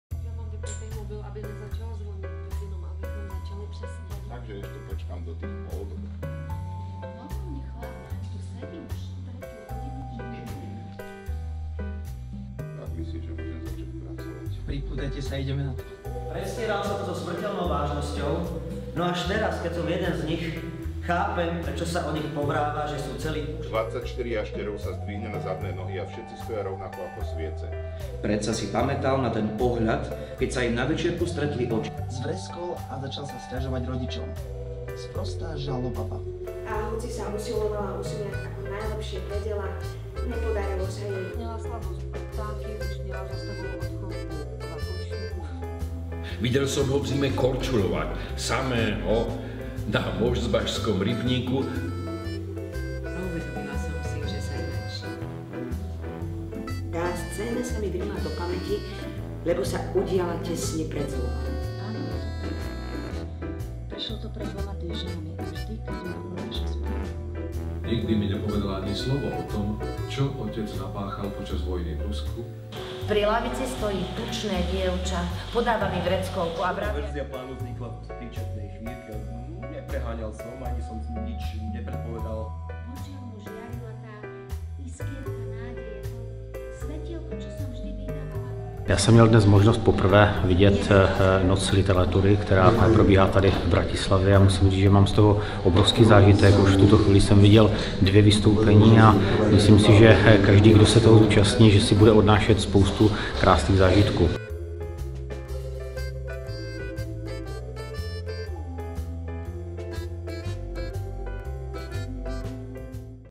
Noc literatúry 2019
Priaznivci kvalitnej literatúry sa stretli 22. mája do večerných ulíc dvadsiatich slovenských miest, kde si vypočuli úryvky čerstvých slovenských prekladov literárnych textov európskych autorov/autoriek na miestach bežne s literatúrou nespájaných. Herci a herečky čítali z románov a poviedok, ktoré sú aktuálne na pultoch kníhkupectiev. Noc literatúry v Bratislave previedla návštevníkov uličkami Starého Mesta, najmä Baštovou a Zámočníckou, ale aj ďalšími. Vo všetkých do Noci literatúry zapojených slovenských mestách sa čítali rovnaké úryvky, ale počet miest čítaní bol rôzny.